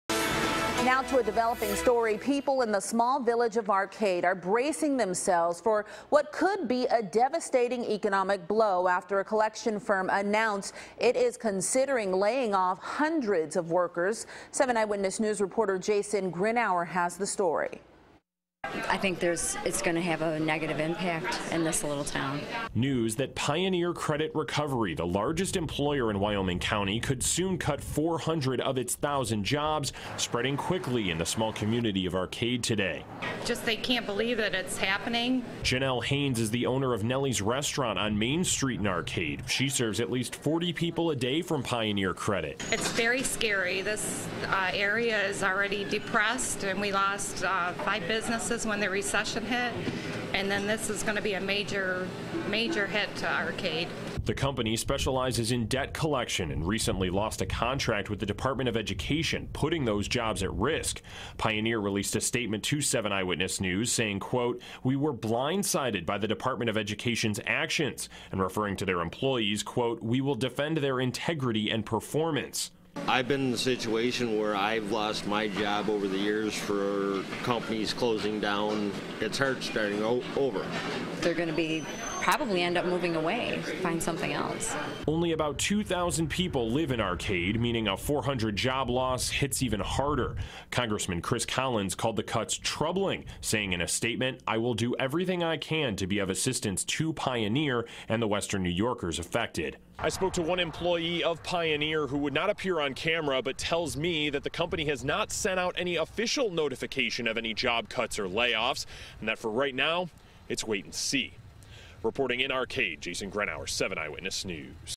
Job cuts threaten major Arcade business
A Wyoming County collection firm could soon lay off hundreds of people in the small Village of Arcade and the company and the community aren't happy about it.

7 Eyewitness News is Buffalo's Source for news, sports, weather, and good things happening every day.